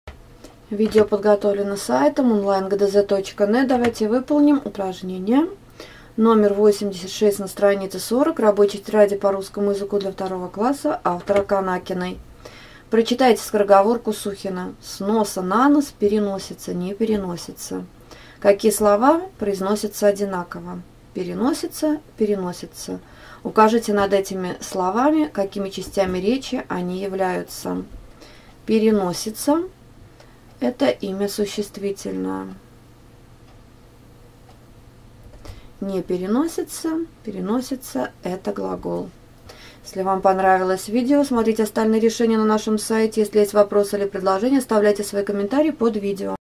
Упражнение 86 - ГДЗ по Русскому языку Рабочая тетрадь 2 класс (Канакина, Горецкий) Часть 2
Выполняя задание №86 на странице 40 вам нужно уметь находить слова, которые одинаково произносятся, указывать над ними часть речи. После просмотра нашего видео, менее чем за 1 минуту можете выполнить данное задание.